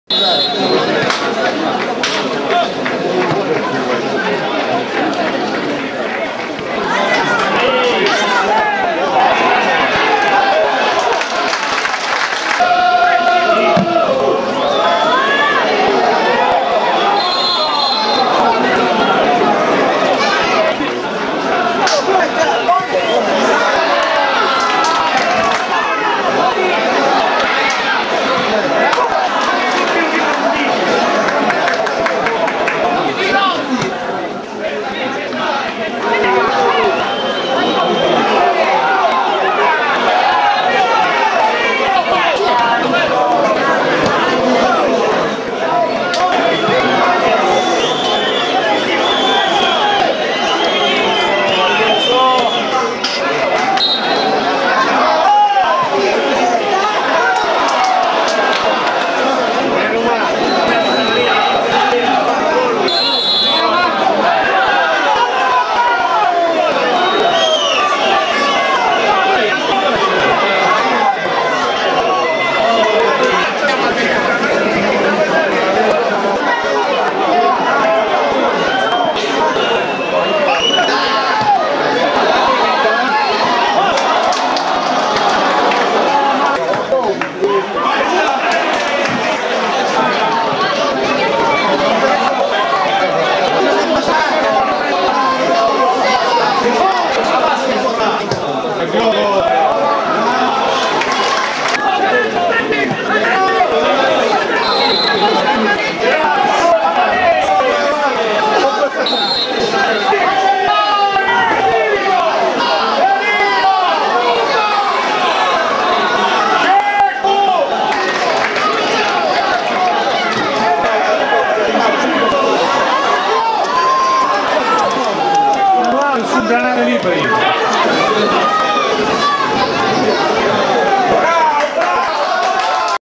Igea Virtus Valle del Giovenco 0 0 07 12 2008
Calcio d’archivio (Lega Pro-Seconda Divisione Girone C): Igea Virtus-Valle del Giovenco 0-0 (07/12/2008)